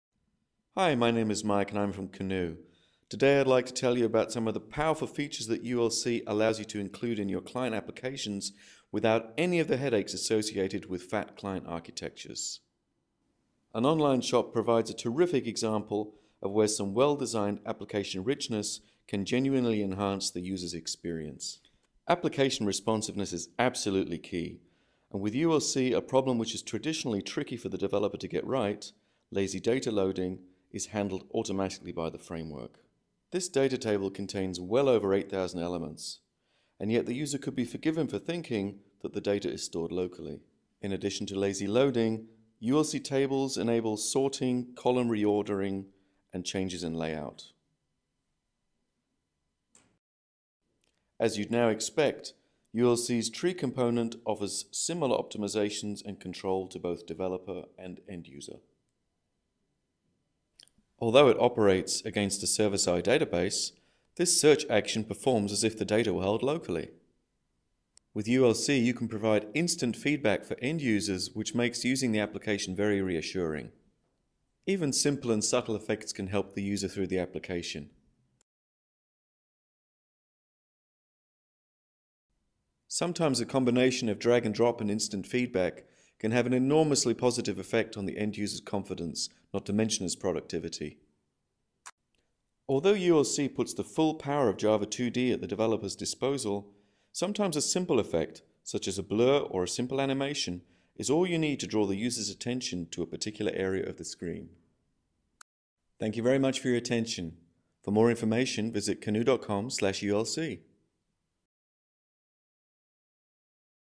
Demo On Rich Internet Applications
How to use a rich internet application (RIA) for simple uses - in this demo, an online shop is built with Canoo UltraLightClient (ULC).